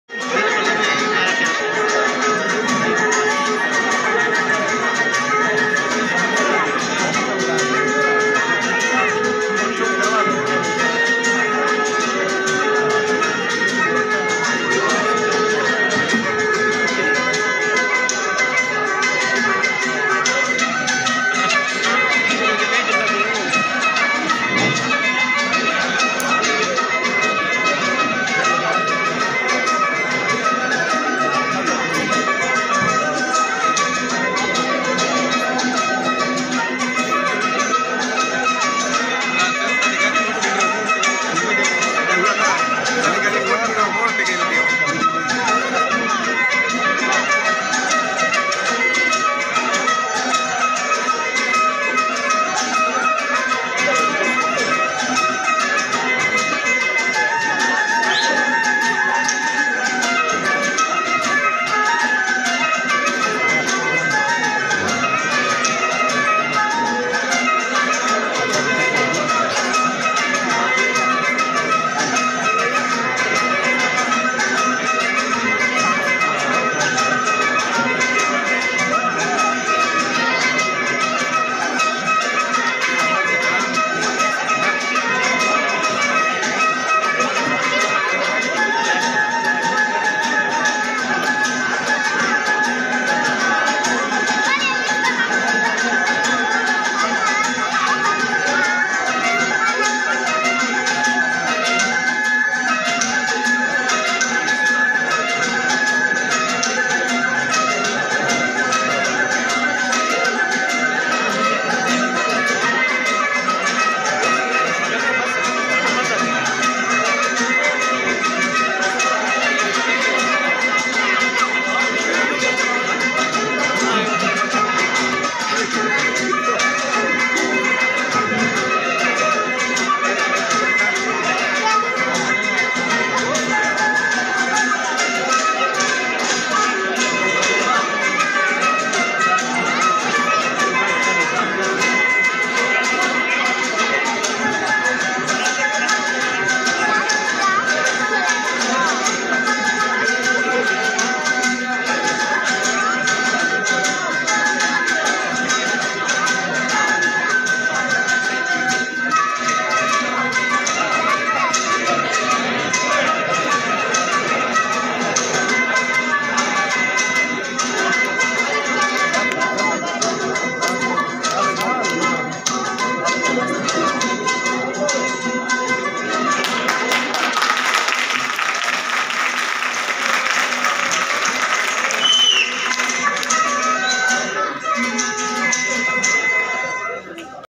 Balouch/Baloch culture day 2 March 2024 function
Baloch culture day is a very interesting and important day for baloch people.
baloch culture day 2024 is not celebrated here but now a days they are celebrating in the world level.